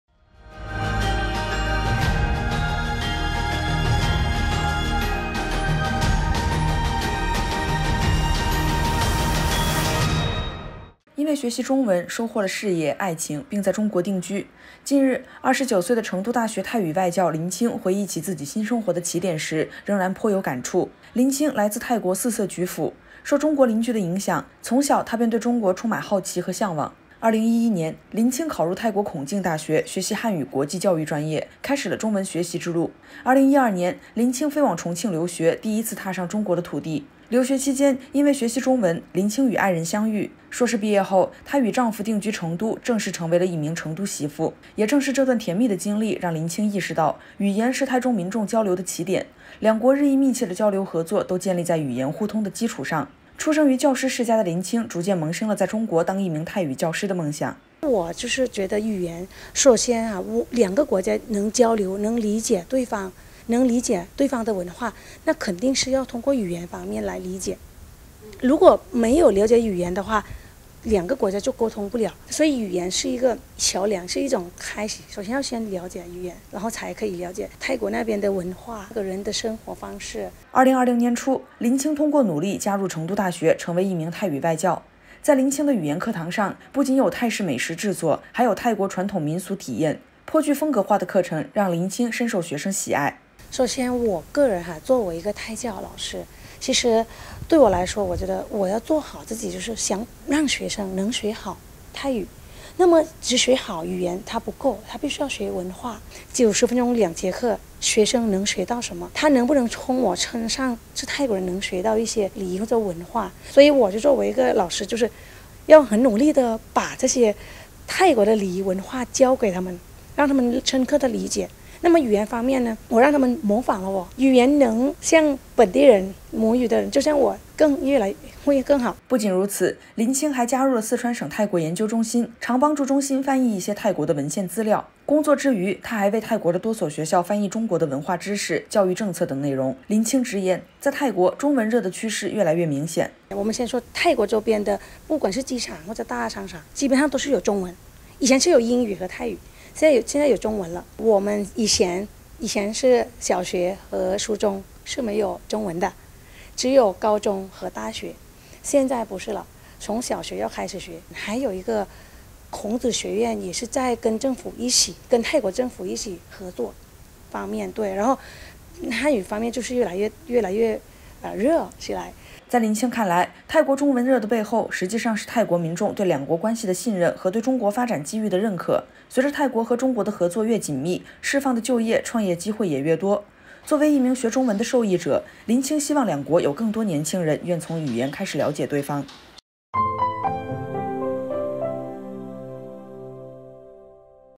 “90后”泰语外教的“中文热”：语言是一个桥梁是一个开始
林青来自泰国四色菊府，受中国邻居的影响，从小她便对中国充满好奇和向往。2011年，林青考入泰国孔敬大学，学习汉语国际教育专业，开始了中文学习之路。2012年，林青飞往重庆留学，第一次踏上中国的土地。留学期间，因为学习中文，林青与爱人相遇。硕士毕业后，她与丈夫定居成都，正式成为了一名“成都媳妇”。也正是这段甜蜜的经历让林青意识到，语言是泰中民众交流的起点，两国日益密切的交流合作都建立在语言互通的基础上。出生于教师世家的林青逐渐萌生了在中国当一名泰语教师的梦想。